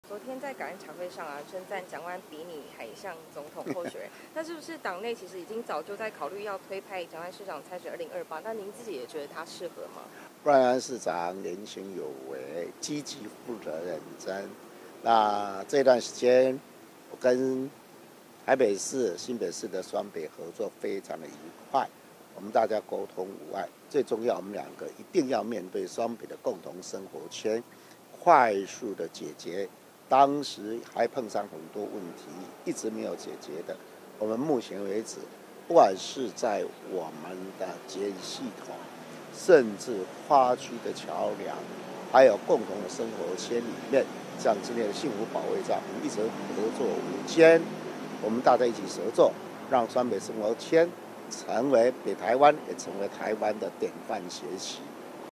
快新聞／笑稱蔣萬安比他還像總統候選人 侯友宜回應了－民視新聞
【民視快新聞】即時中心／顏一軒報導

國民黨總統落選人、新北市長侯友宜昨（26）日在黨主席朱立倫、台北市長蔣萬安的陪同下，一起在中央黨部舉行台北謝票茶會，會中他打趣提及，「蔣市長全台灣走透透，而且又是帥哥，站他旁邊，想說他變成總統候選人」引發討論。對此，侯友宜今（27）日也做出最新回應。